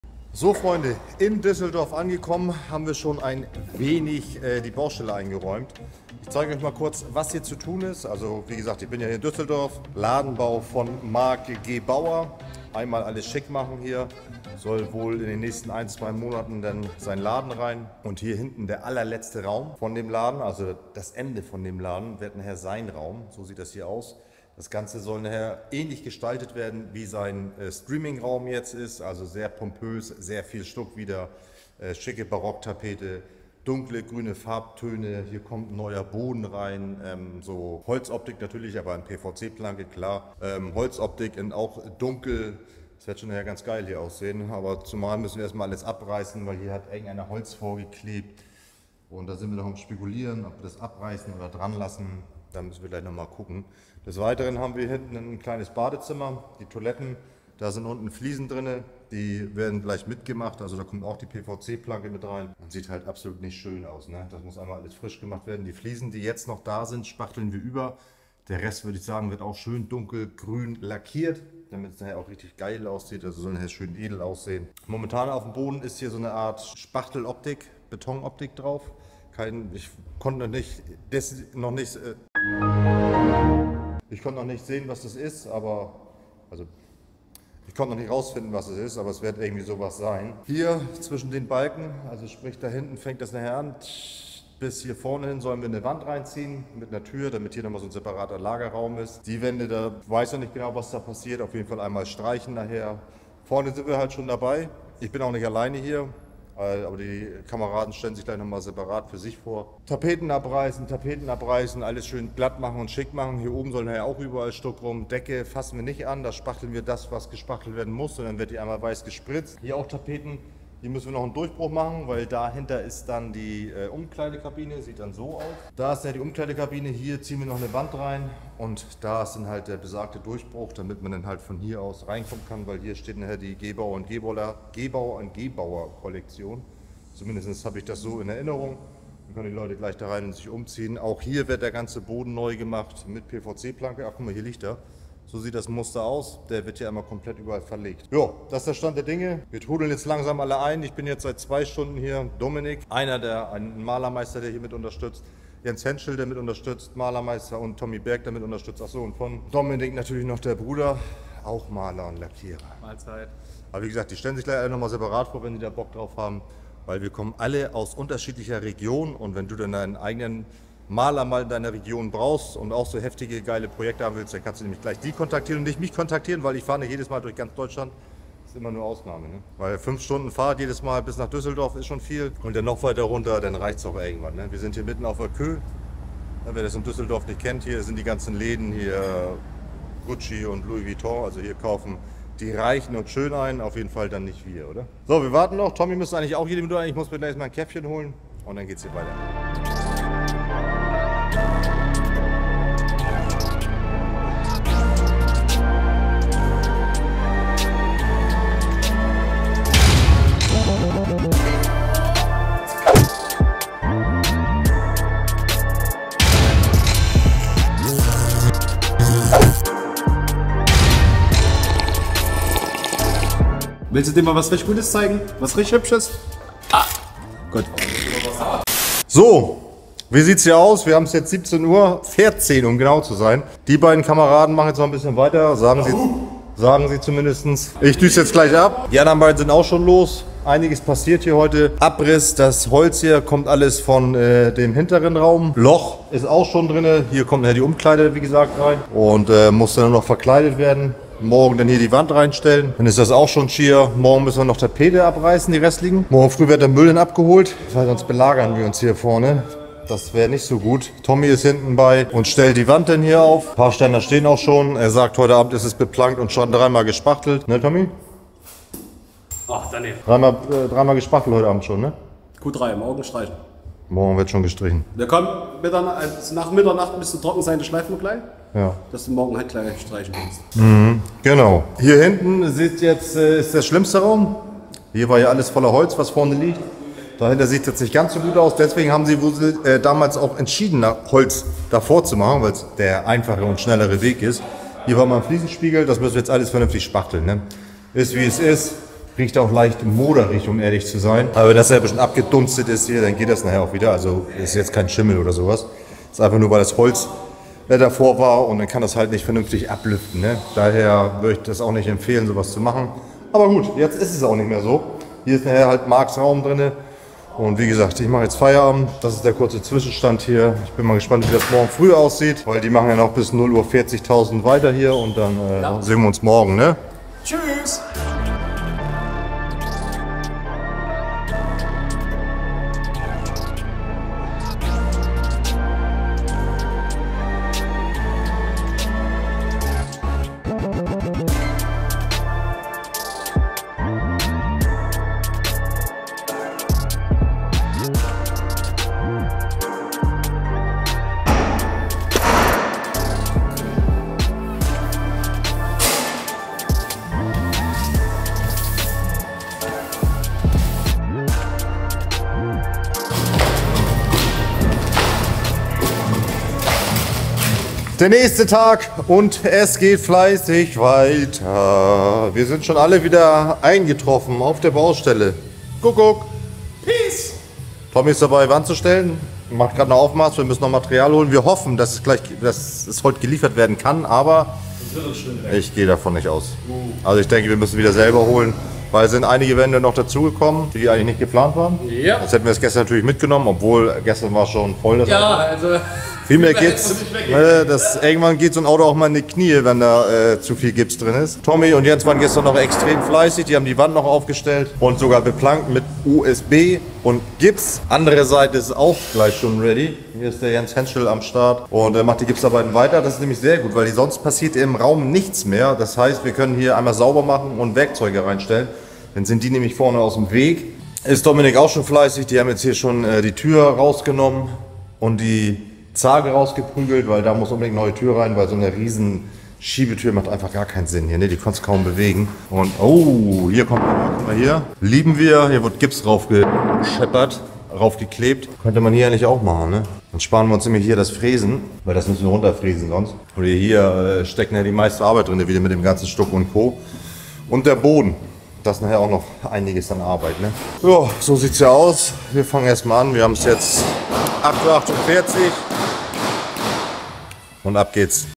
Ladenbau für @marc.gebauer in düsseldorf
Endlich geht es weiter mit einem neuen Blog, wir sind in Düsseldorf und fangen an den Ladenbau für Marc Gebauer an, um zu gestalten. Viel Spaß bei dem Video.